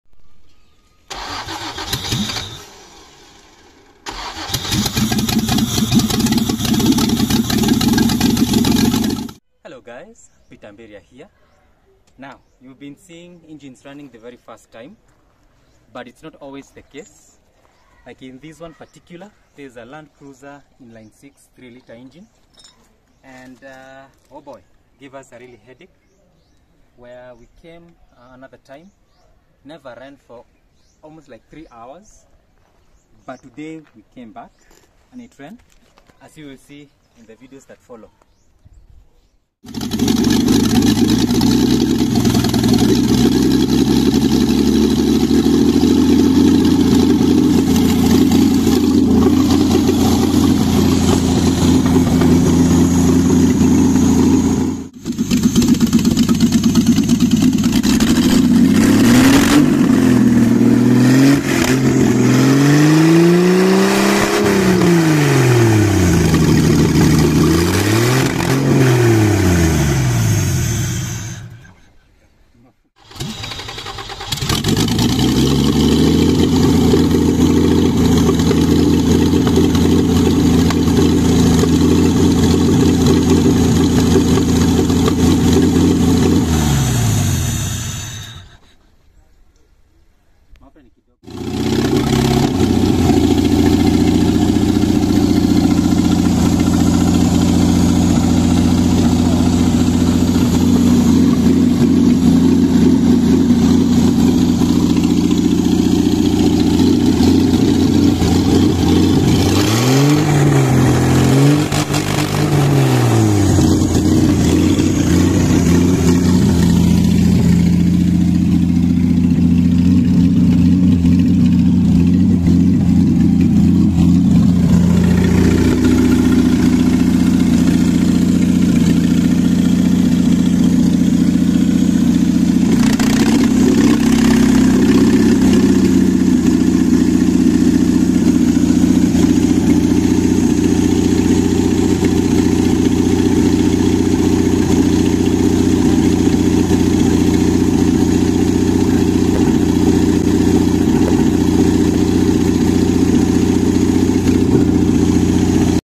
A rough start, quite literally!!
Dear engine lovers fraternity!!

These engines can really make someone be humble by default.

This engine in particular was already EFI but with the first gen analog ECUs. We built a fresh harness and converted it to use Voltarent ECU. We had a rough start where on the first day, it declined to run completely. It would appear as if it's about to start but never starts.

We had removed the first spark plug which was iridium and assumed the rest are the same, shock on us. On the second day, after trying to start it in vain, I thought, why not remove the rest and check their condition.

Well, on this 6 cylinder engine, there were like 4 brands of plugs!!

Since we didn't have replacements, we just cleaned them and the engine started right away. The rest, enjoy the sound!!!